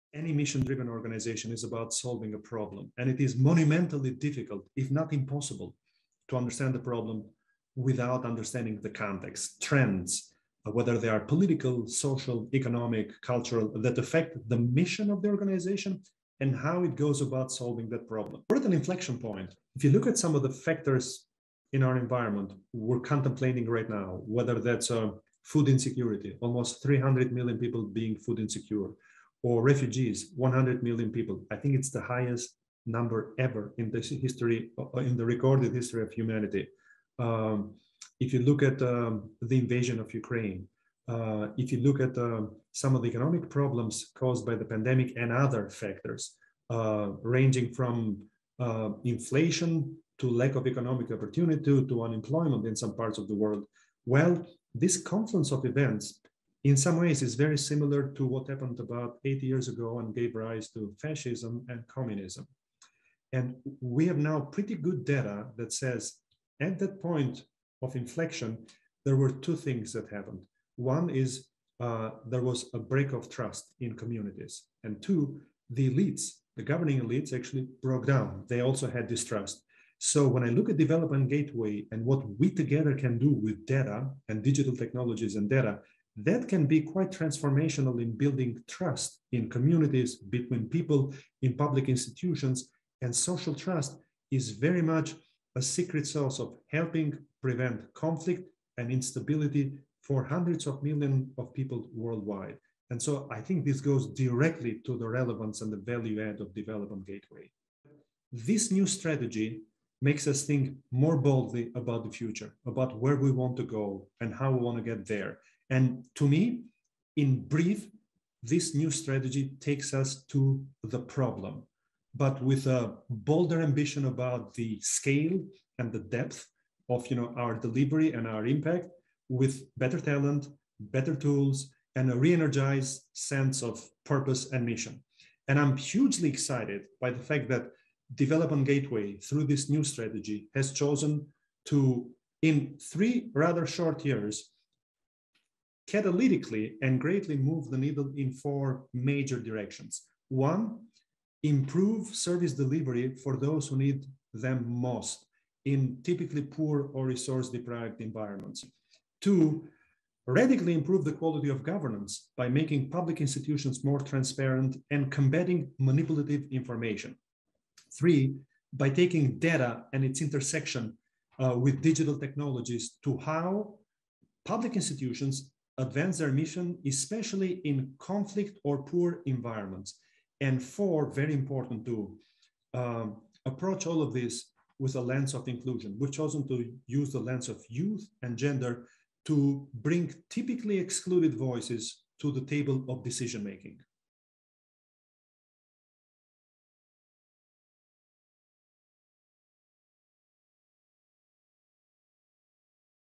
Developing a Strategic Plan in the Current Global Moment
We talked to Aleks Dardeli, Chair of the DG Board and Executive Vice President of IREX about developing DG's new Strategic Plan. Throughout the conversation, Aleks highlighted the importance of the current moment in global affairs. Between the largest youth population in human history, recent democratic backsliding, societal changes from the COVID-19 pandemic, and more, now is a crucial moment to reassess how we can best use data and digital to solve problems.